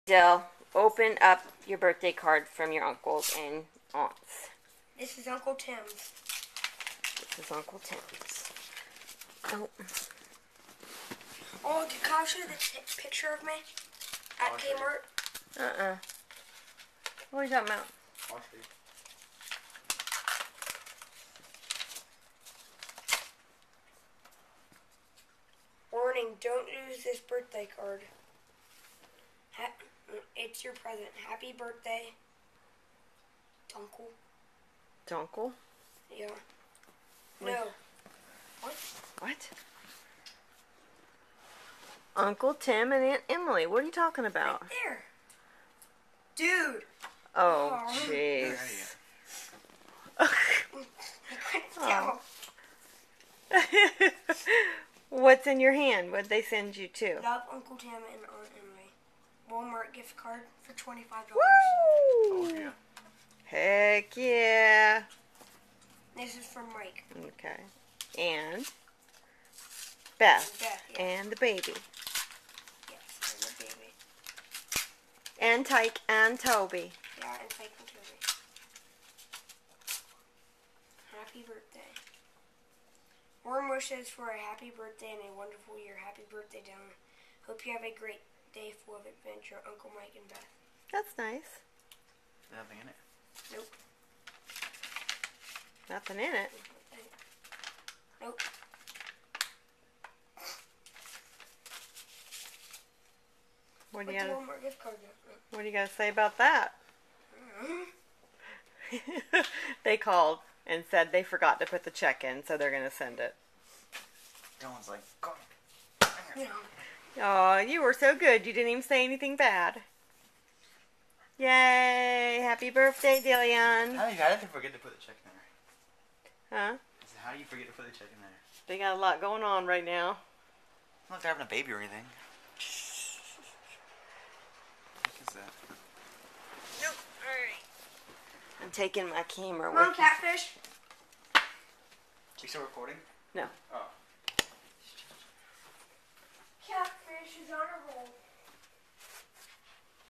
Card Opening 2012.MP4
Dillon opening his cards from his aunts & uncles.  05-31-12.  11th birthday